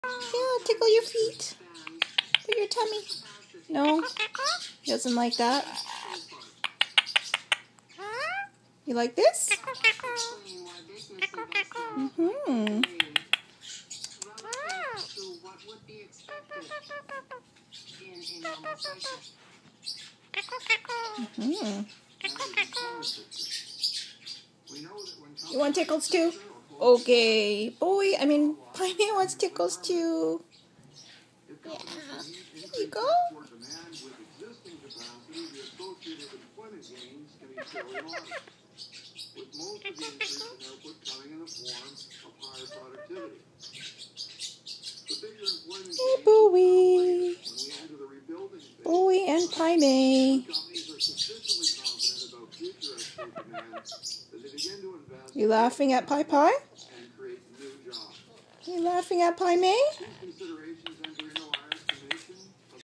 Birdy Mocking Doggy
I get the feeling Bowie feels entitled to  tickles. Haha.
& don't worry, even tho Paimei looks desperately jealous, he get a LOT of his own tickles, hugs & kisses from me! Bowie is an Indian Ringneck & PaiMei is a Maltese.